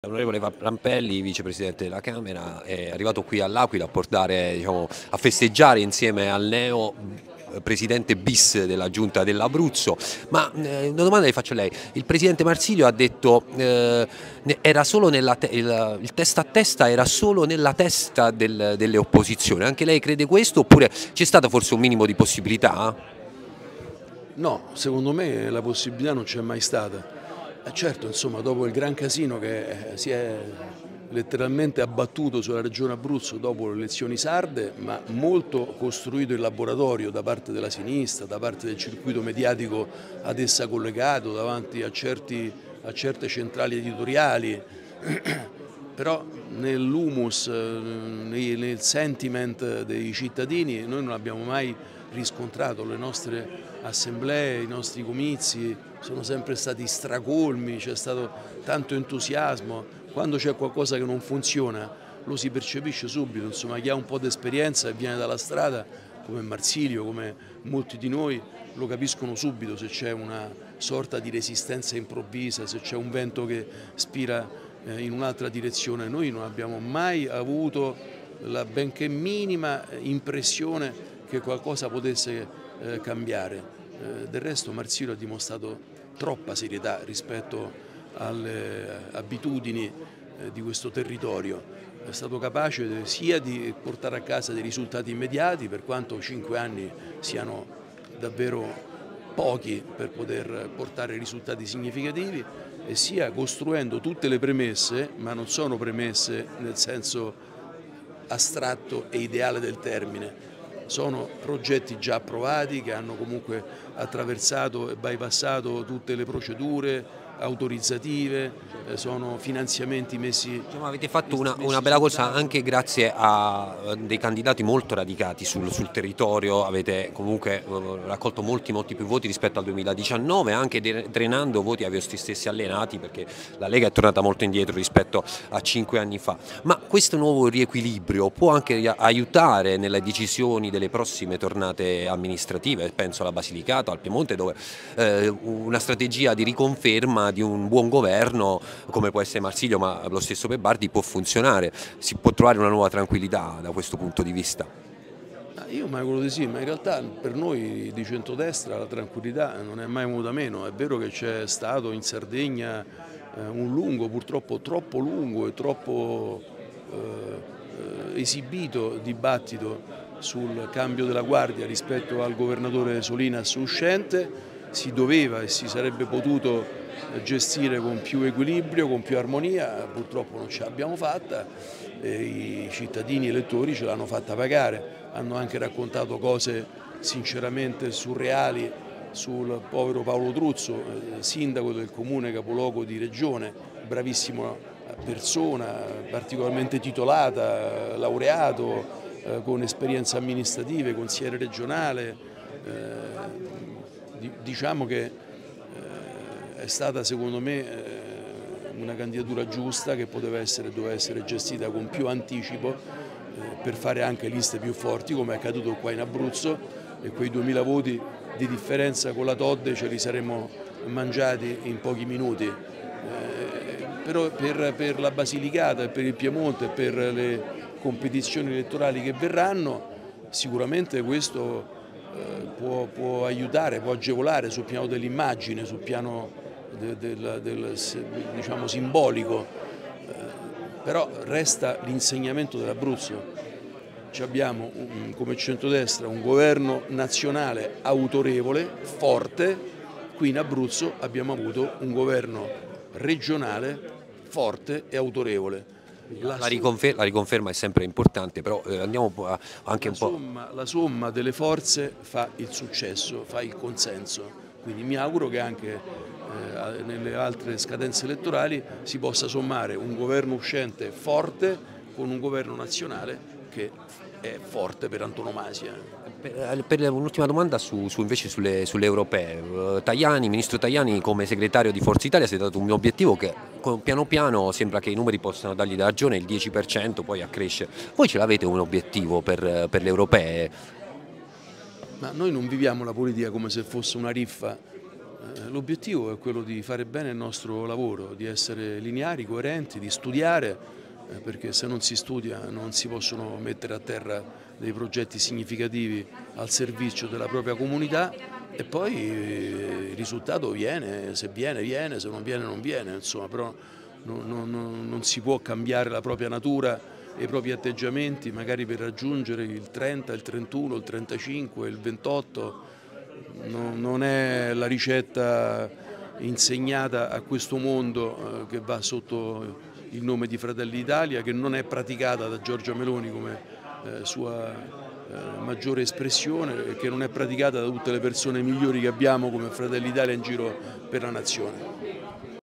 Elezioni Abruzzo, Rampelli festeggia la vittoria di Marsilio: “Mai avuto dubbi su questa vittoria”
Elezioni Abruzzo, Rampelli festeggia la vittoria di Marsilio: “Mai avuto dubbi su questa vittoria, forse in Sardegna avremo dovuto anticipare le scelte”